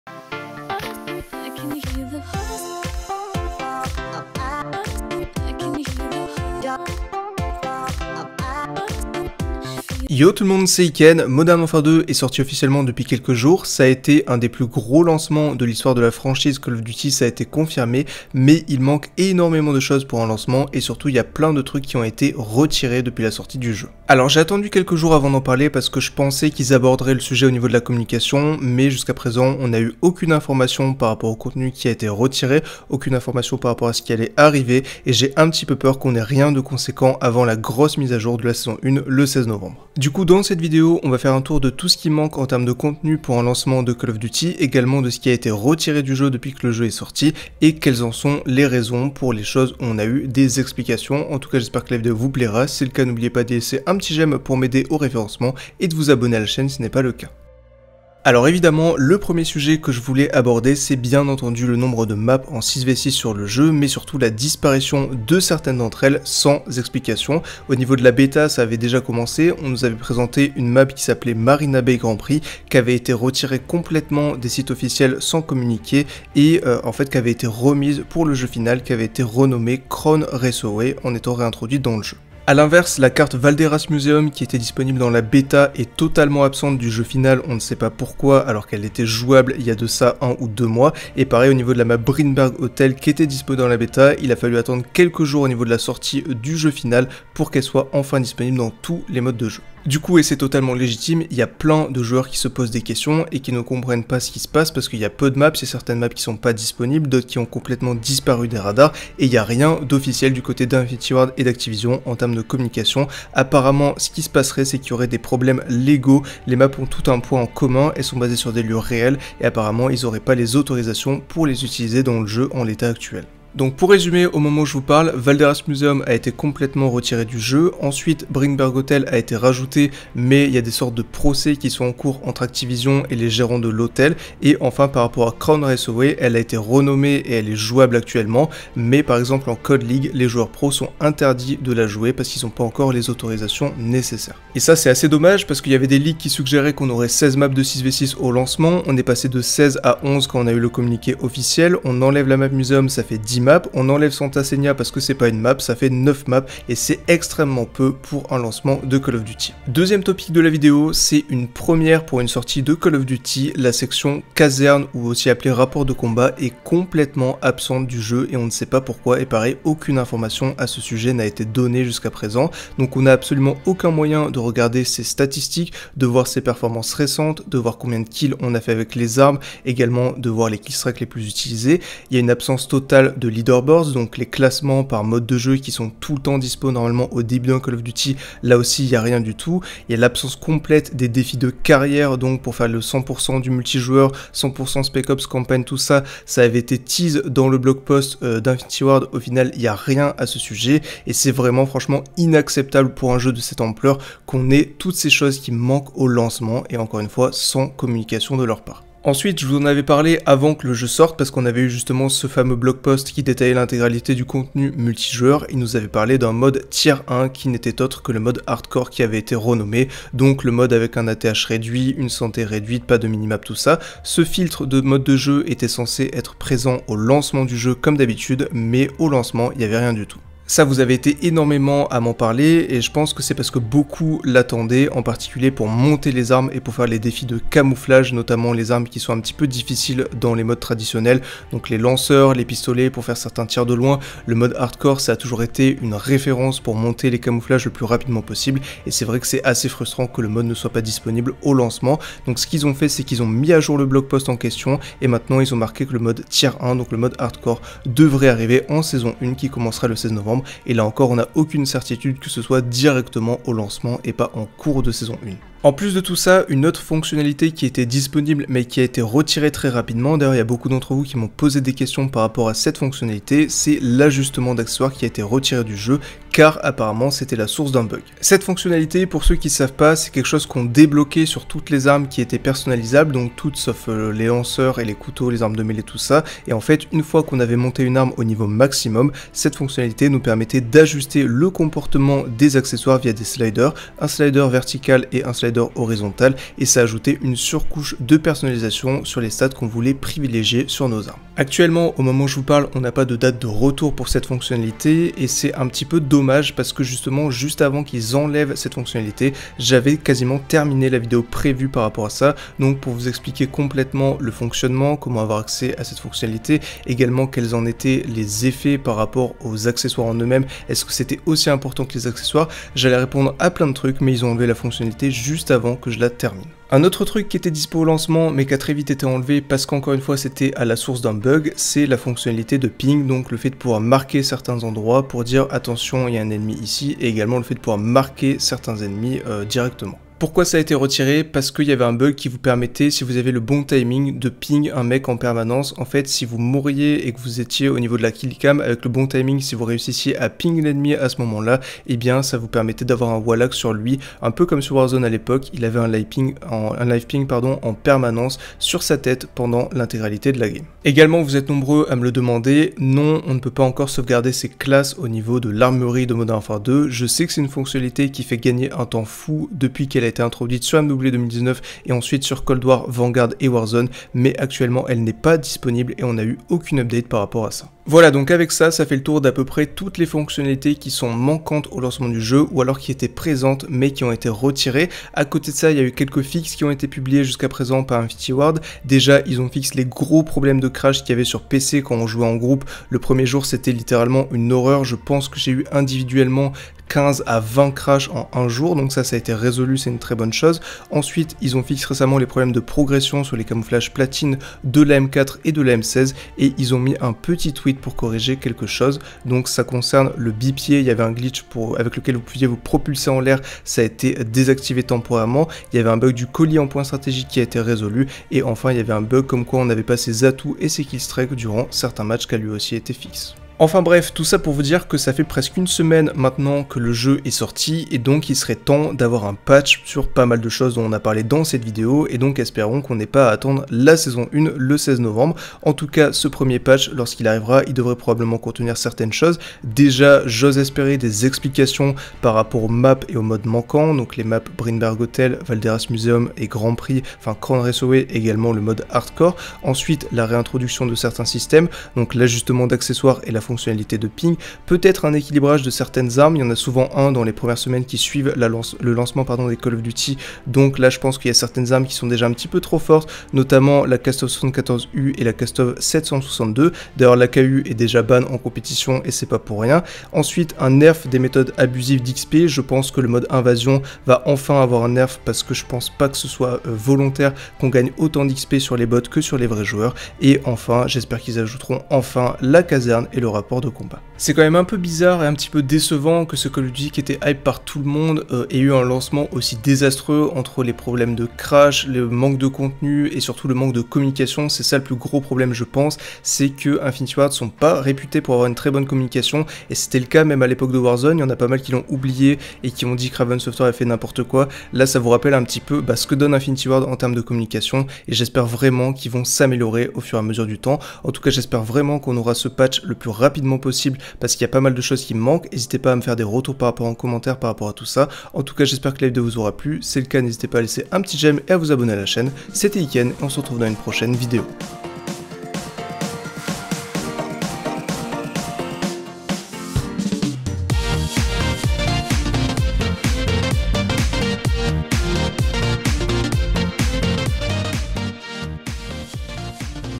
Les Patchs de Modern Warfare 2 Ont Supprimé Toutes Ces Choses... (Maps, Modes, Fonctionnalités)
Le lancement de Modern Warfare 2 a été assez chaotique, et marqué par de nombreux bugs. Pire que ça, il manque énormément de fonctionnalités de base, et pas mal de contenu a été repoussé voire complètement retiré du jeu : des maps, modes de jeu, etc... Aujourd'hui on fait le point sur tout ce contenu manquant, et on anticipe l'arrivée du premier gros patch de MW2.

00:00 Introduction
00:57 Les Maps Manquantes (Problèmes Légaux)
02:39 Où est le Rapport de Combat ? Les Stats ? Les Défis ?
03:29 Le Mode Hardcore est Repoussé à la Saison 1
04:25 L'ajustement d'Accessoires Retiré Temporairement de MW2
05:26 Retrait du Système de Ping
06:00 Comment Sauvegarder ses Classes sur MW2 ?
06:18 Les Premiers Patchs d'Infinity Ward
07:03 Le Gros Patch qui Devrait Arriver avant la Saison 1
08:04 Conclusion